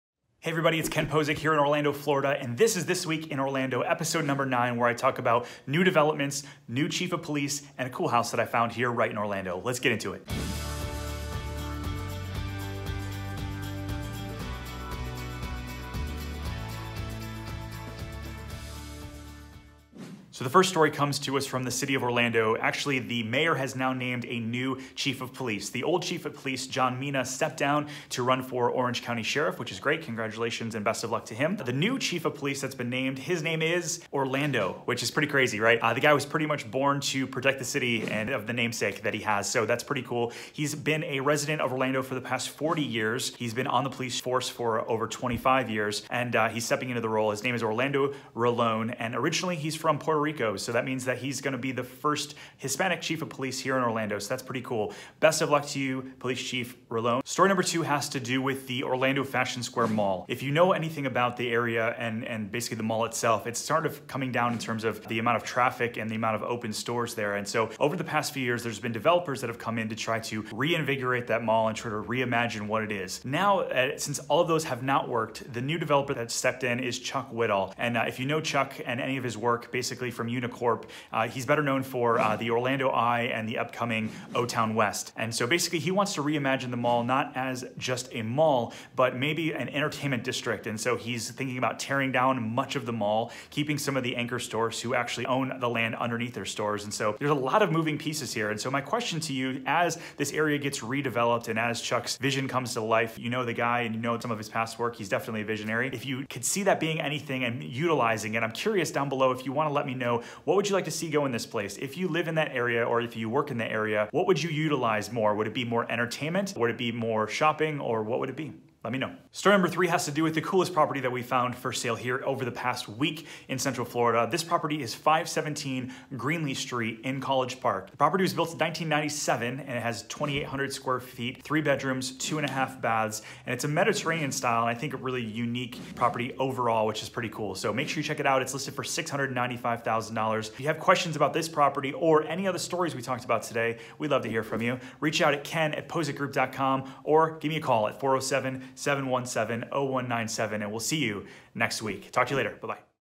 New Orlando Police Chief | Fashion Square Mall Reimagined | EP 9 of The Week In Orlando!
There's a new Orlando police chief after chief Mina stepped down to run for Orange County Sheriff.

A prominent developer is reimagining the Fashion Square Mall

PLUS one of the coolest homes I've found recently listed here in Orlando. Take a look!